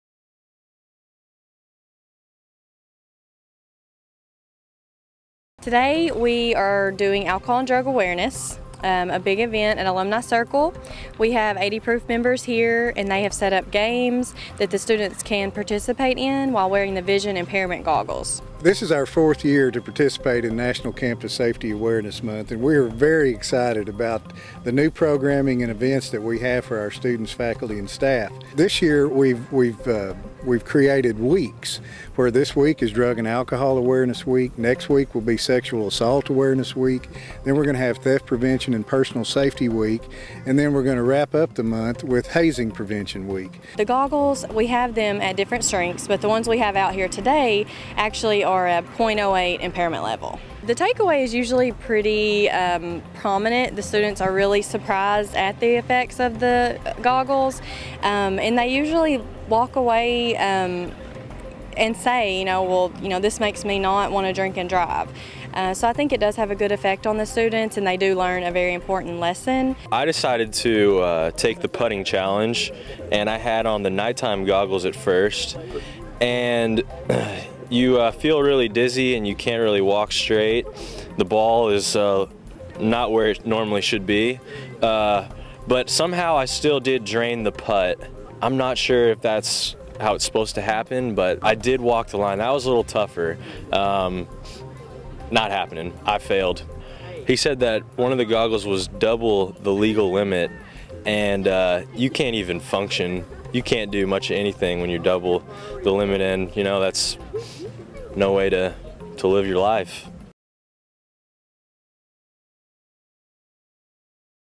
80 Proof Carnival kicks off UCA's National Campus Safety Awareness Month 2013
Members of the RSO, 80 Proof coordinated games such as putt putt
golf, bowling, and ping pong for students to participate
in while wearing vision impairment goggles.